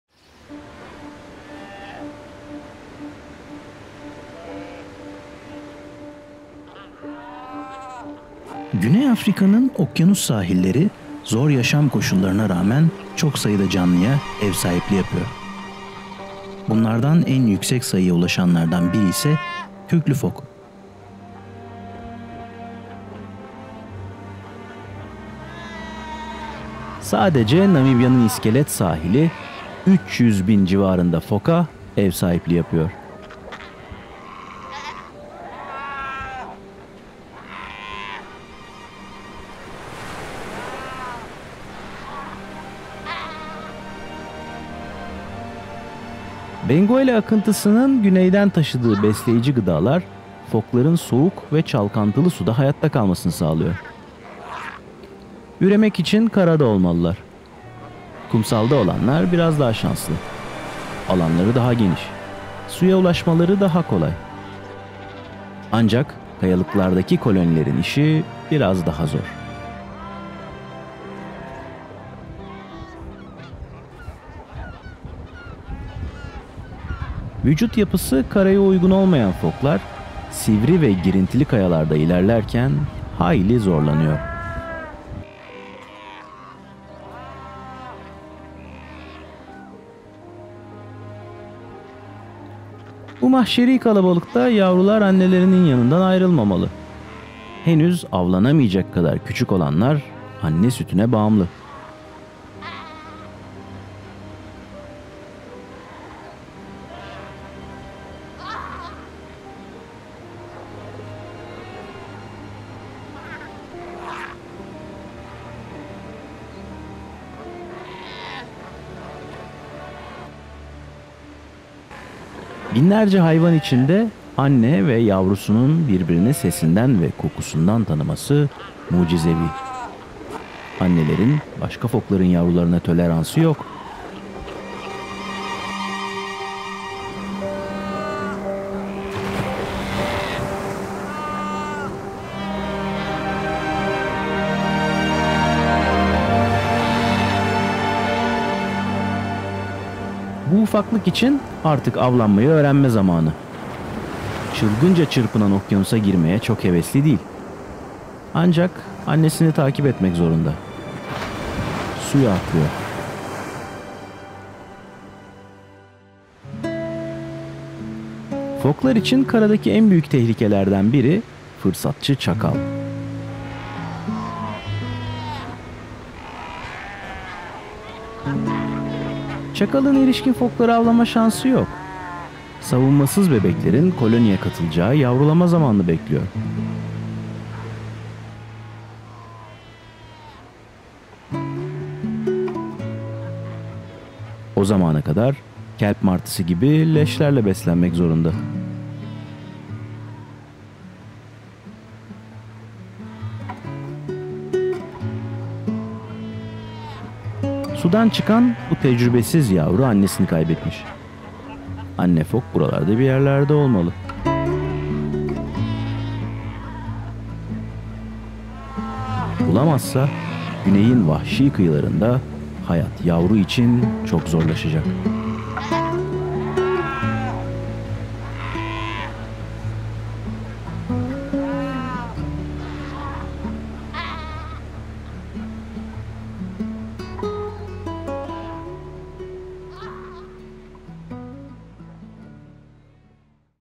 Namibya Kıyılarında 300.000 Kürklü Fok
Afrika'nın güneyinde, Botsvana, Namibya ve Güney Afrika Cumhuriyeti'nde çektiğim görüntülerden yaptığım kısa belgesellerin üçüncüsü kürklü foklar.
Yüz hatları köpeği andırdığı için "deniz köpeği" lakaplı foklar Afrika kıyılarında yüksek sayılara ulaşıyor. Bazı koloniler kumsalda, bazılarıysa kayalıklarda yaşıyor. Suda oldukça çevik ve hareketli olan fokların karada işi çok zor. Üst üste, dip dibe yaşadıkları yetmiyormuş gibi bir de karaya uyumsuz vücutları yüzünden bazen oldukça zorlanıyorlar.